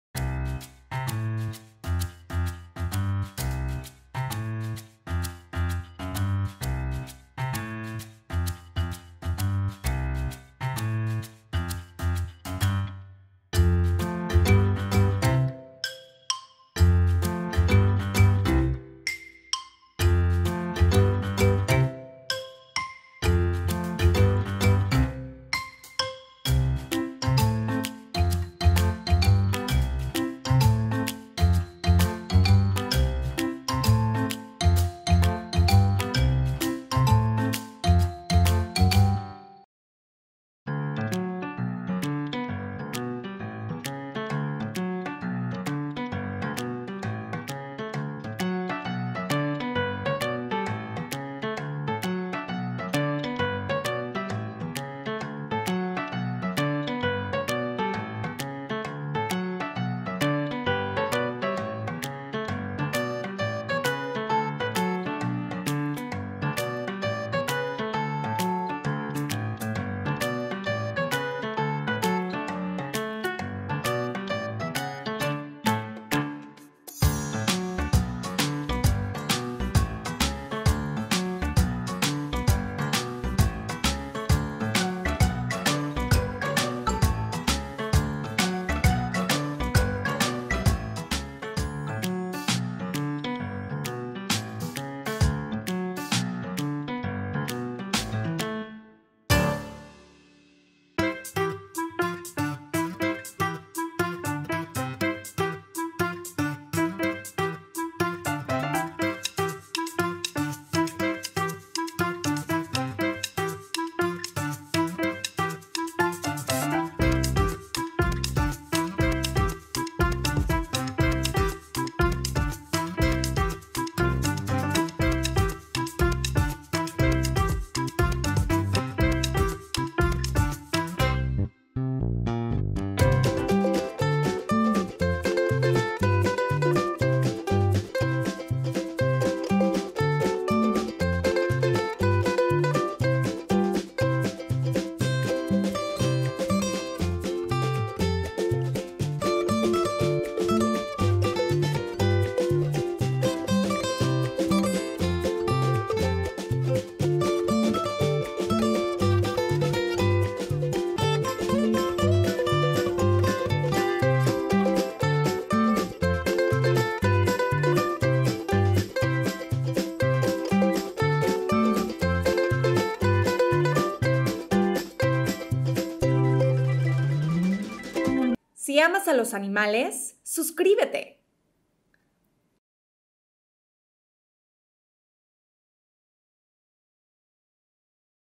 Este Golden Retriever hace todo lo que hace su papá | El Dodo
Este Golden tiene que hacer todo lo que su papá hace, sin importar lo que sea.